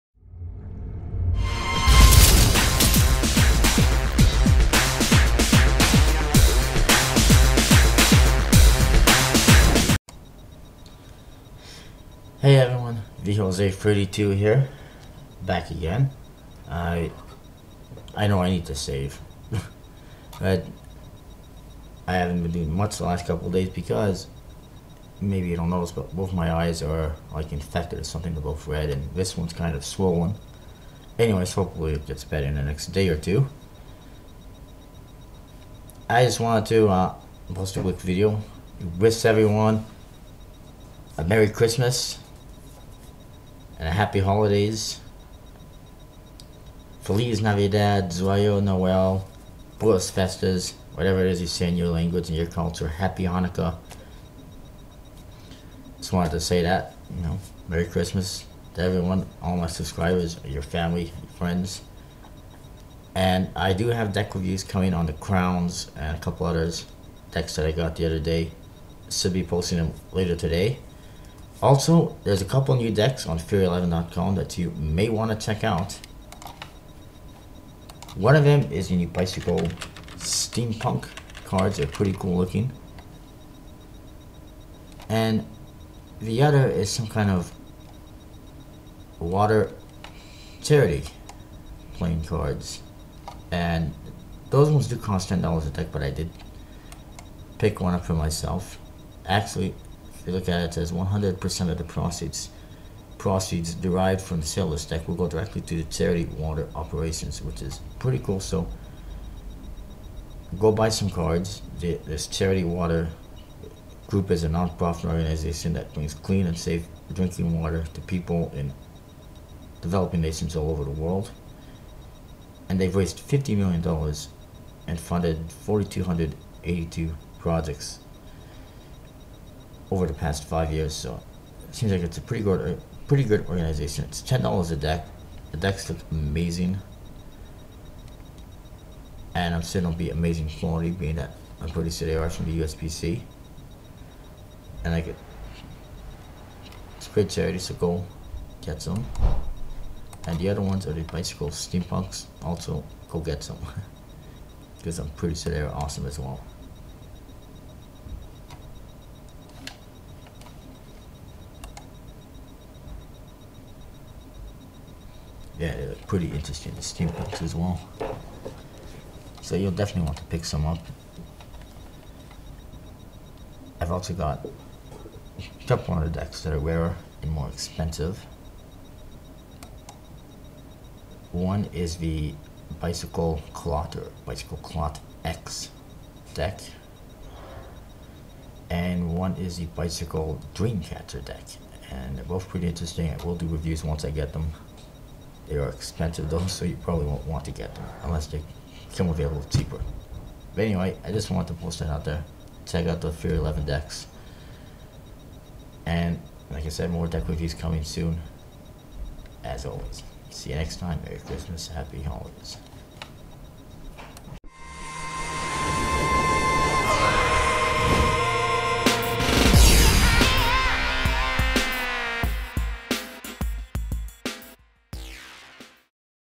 It's Christmas! Theory11 & The Blue Crown Have Decks For You!
Merry Christmas and Happy Holidays to Everyone! Also, please check out Theory11 for their 2 new decks - Bicycle Steampunks and Water Charity cards (it's for a good cause), also didn't mention, but I found out that TheBlueCrown.com will be releasing more Black Crowns on Monday!